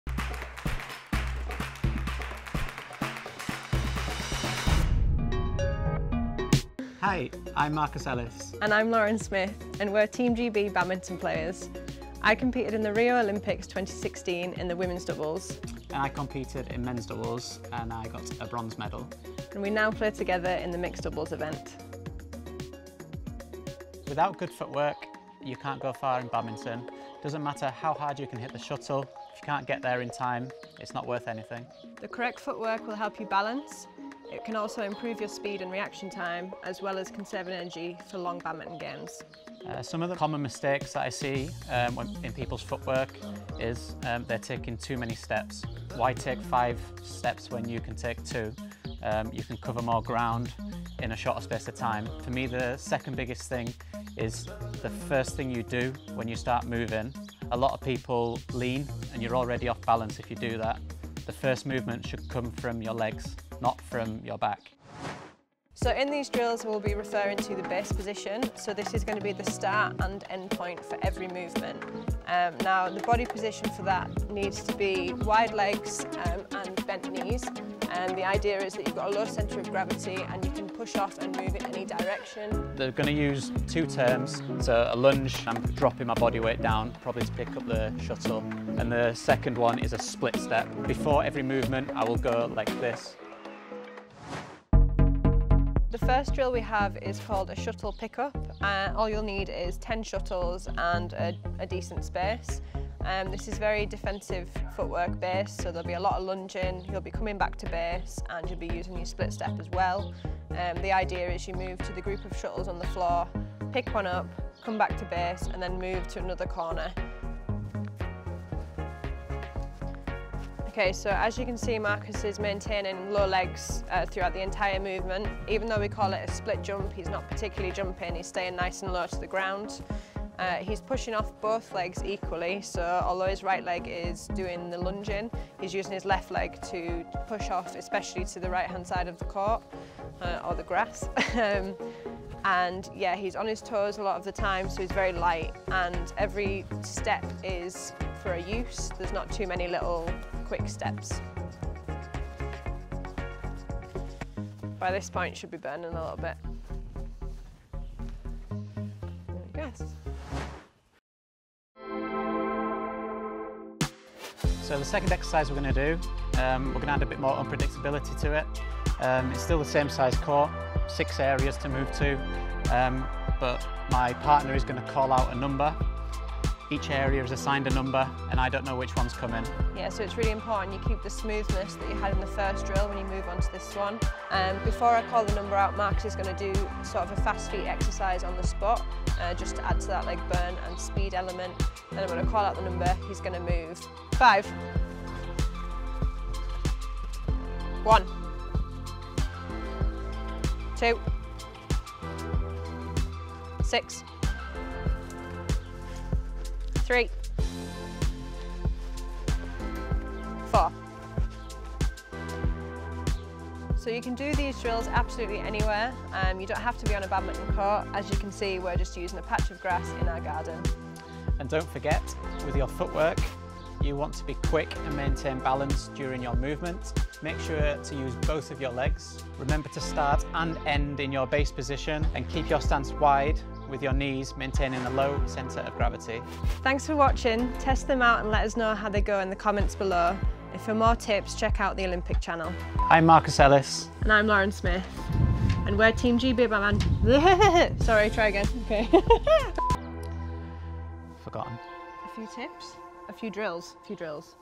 How to Improve your footwork for Badminton ft. Lauren Smith & Marcus Ellis | Olympians' Tips
Watch Olympic Bronze Medallist Marcus Ellis and Olympian Athlete Lauren Smith showing you how to improve your footwork in badminton with three drills! The first drill is about your technique, followed by the Shuttle pick-up drill, and last but not least the 6 corners drill.

Don't forget:
1) Be quick and maintain balance
2) Use both of your legs
3) Start and end in your base position
4) Keep your stance wide

Learn how to improve your game from the best athletes in the world with Olympians' Tips